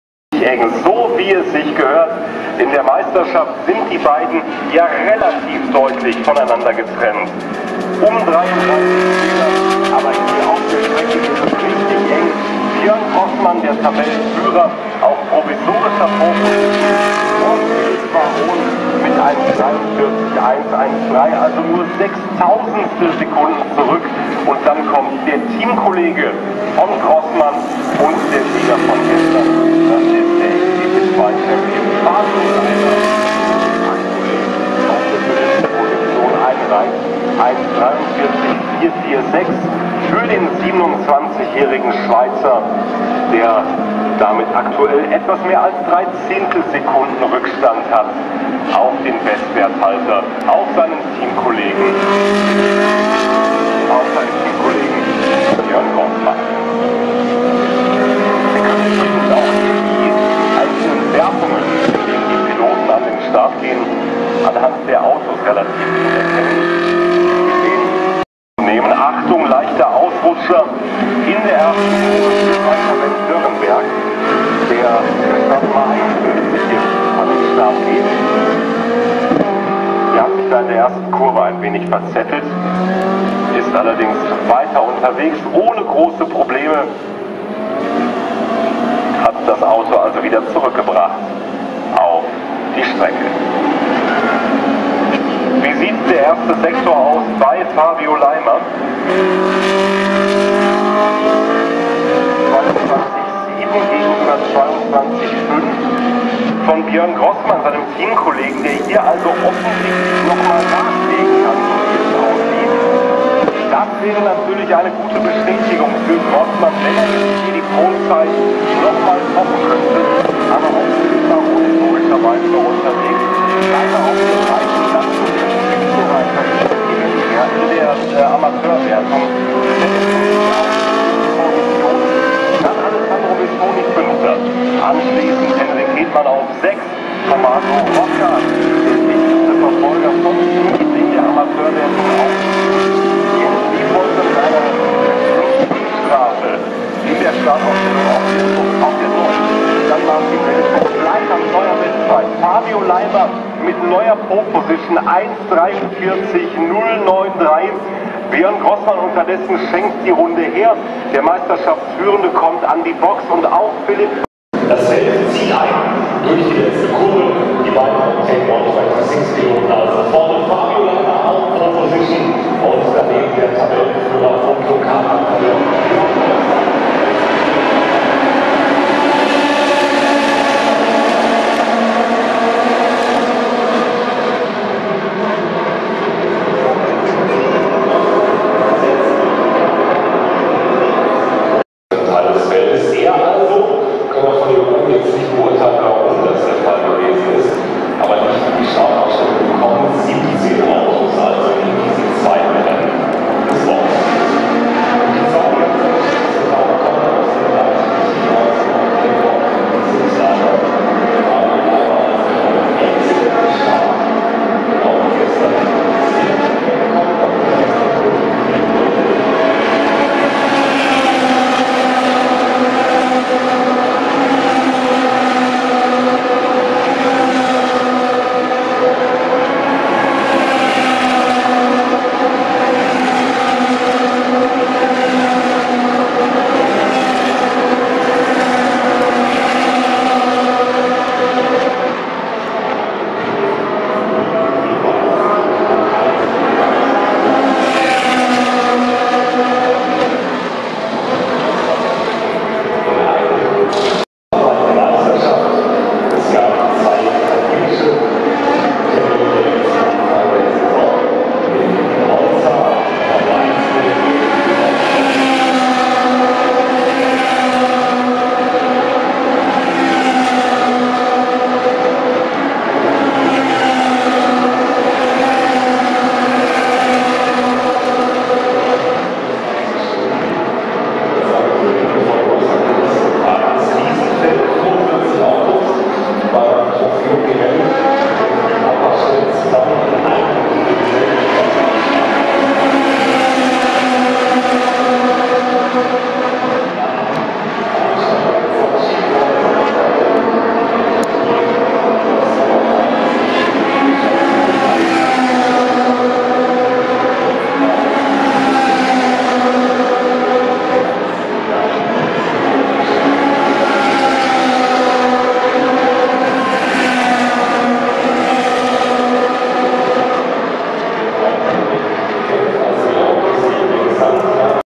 Ferrari Racing Days 2016 in Hockenheim: Ferrari Challenge Trofeo Pirelli am Sonntag
Habe erst die Qualifikation mit Blick auf die Sachskurve, dann das Rennen am Nachmittag von der Haupttribüne aus gefilmt. Viel Spaß. :)